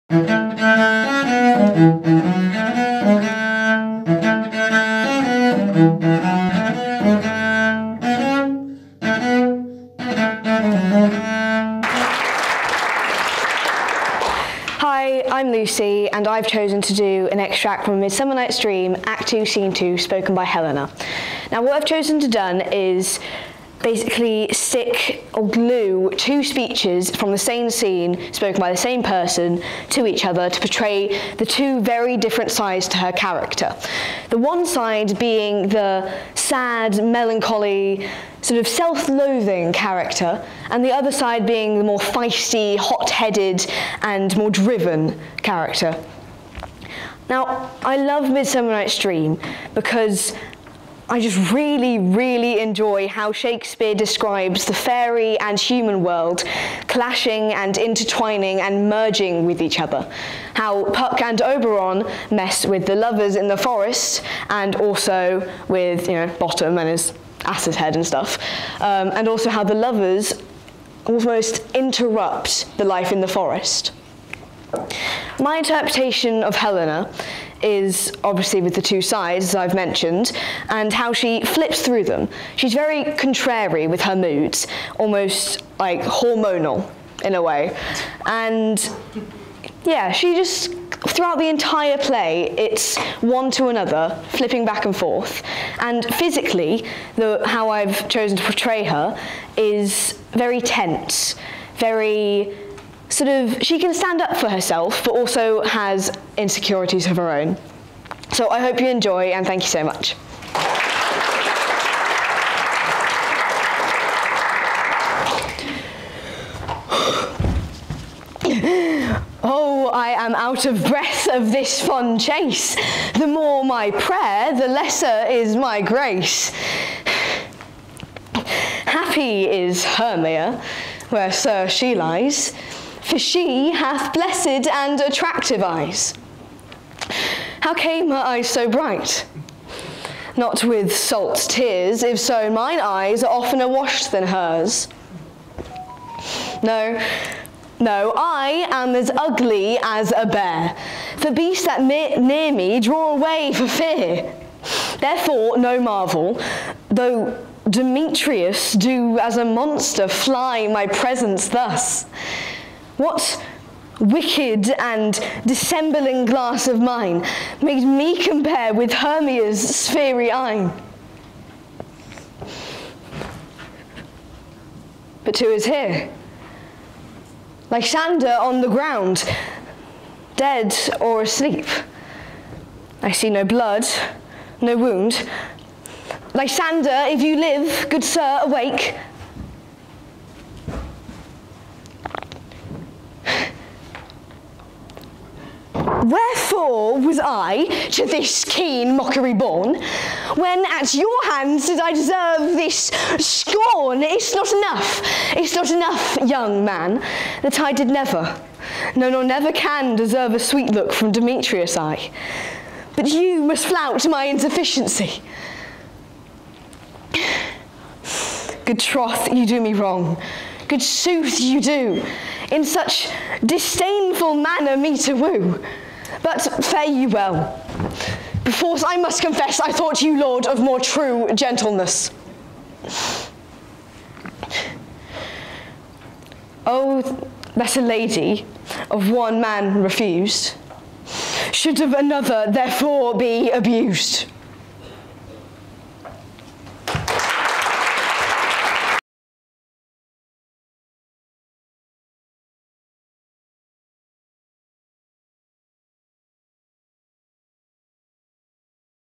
Performing Shakespeare 2022 Grand Final | Stratford Girl’s Grammar School
Lucy from Stratford Girl’s Grammar School performs Act III, scene ii from A Midsummer Night’s Dream at Shakespeare’s Globe in London. Find out more about the annual competition